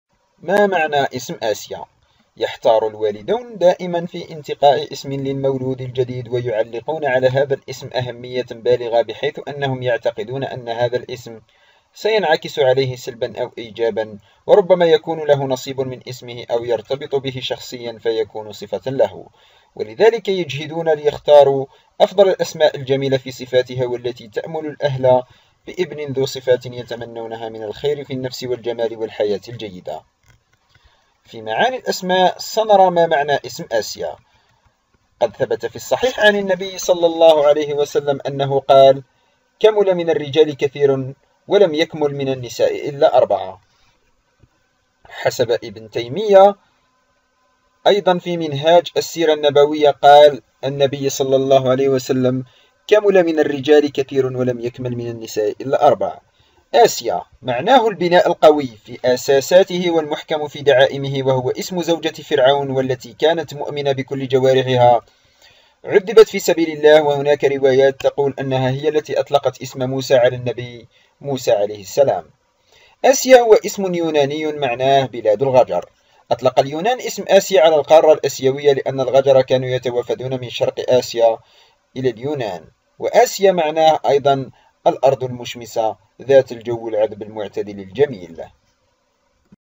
ما معنى اسم اسيا ؟
آسيه، معناه : البناء القوي في اساساته والمحكم في دعائمه، وهو اسم زوجة فرعون والتي كانت مؤمنة بكل جوارحها عذبت في سبيل الله وهناك روايات تقول أنها هي التي أطلقت إسم موسى على النبي موسى – عليه السلام - . آسيا : اسم يوناني ، معناه : بلاد الغجر ، أطلق اليونان إسم آسيا على القارة الآسيوسية لأن الغجر كانوا يتوافدون من شرق آسيا إلى اليونان. وآسيا : معناه أيضا الأرض المشمسة ذات الجو العذب المعتدل الجميل